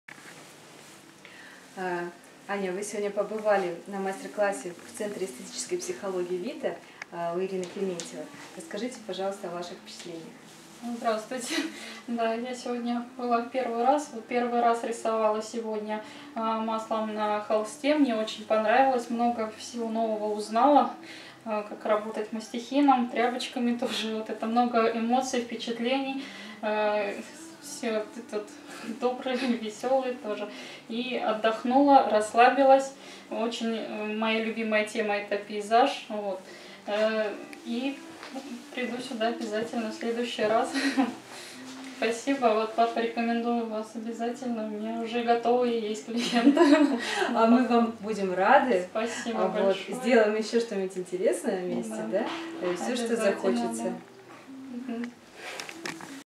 Отзыв Анны о мастер классе живописи маслом на холсте
Отзыв Анны  о мастер классе Художника Ирины Клементьевой IrinaArt.su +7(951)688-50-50 Живопись маслом на холсте. Занятия в Центре Эстетической Психологии ВИТА в Санкт-Петербурге.